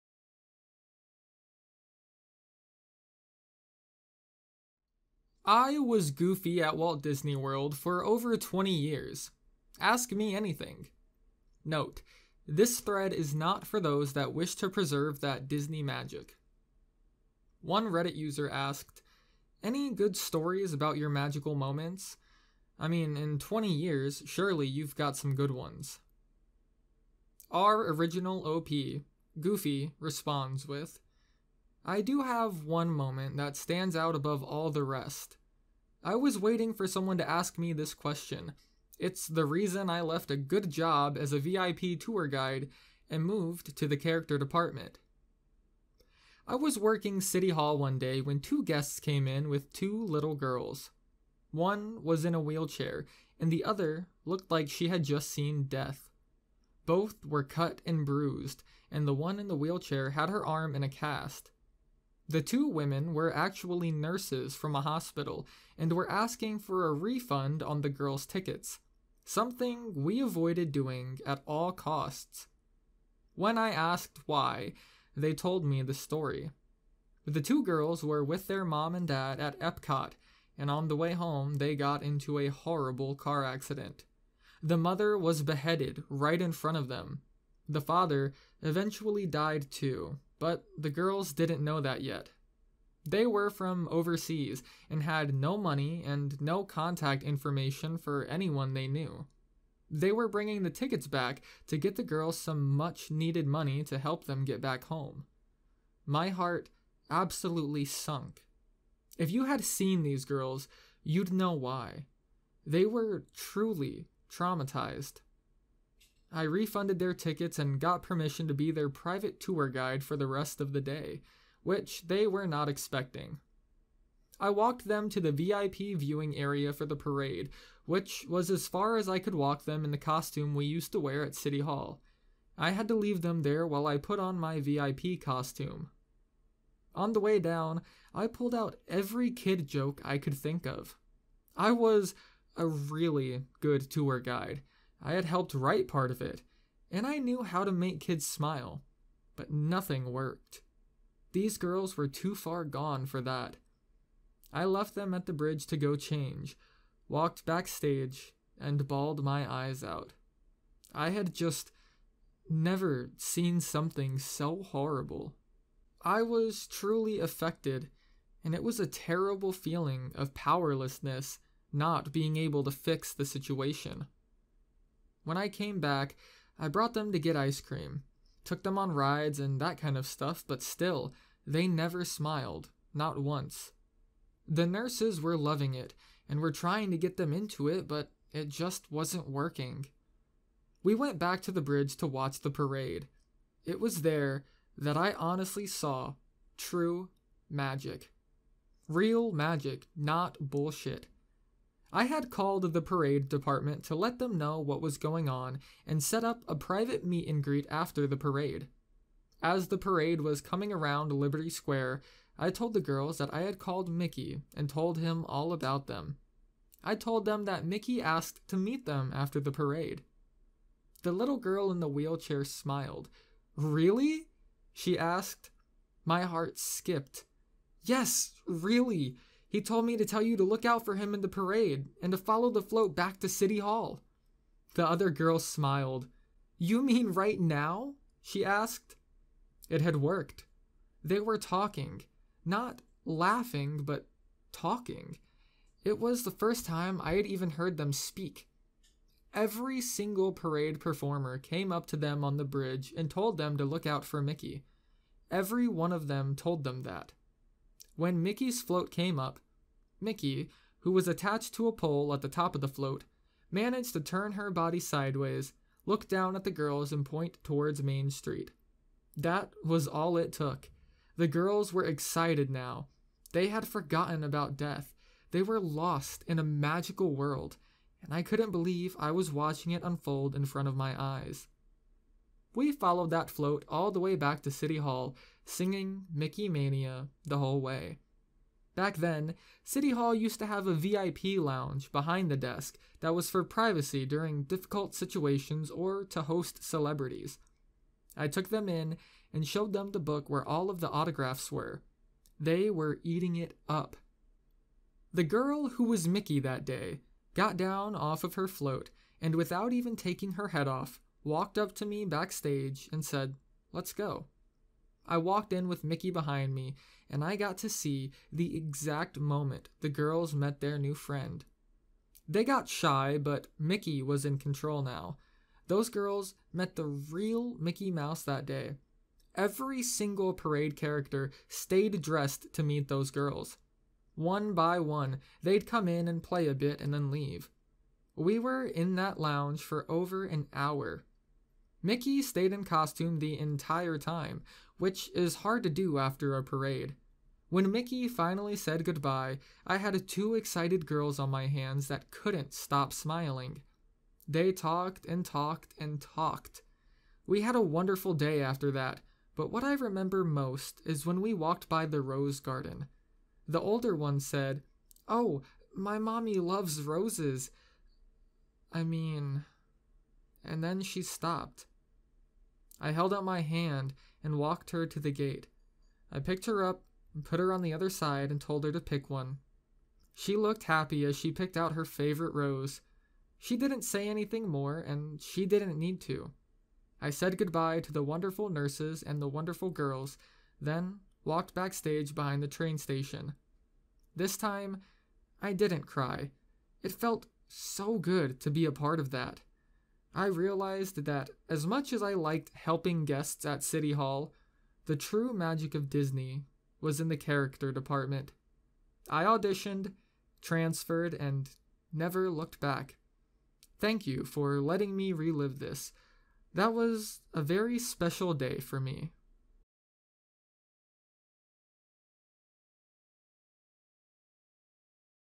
Anon's Wholesome Experience At Disney World - Feels - Reddit AMA
Anon recounts his wholesome experience at Disney World, what made him become Goofy, and hits us with some heavy feels in this Reddit AMA Response thread.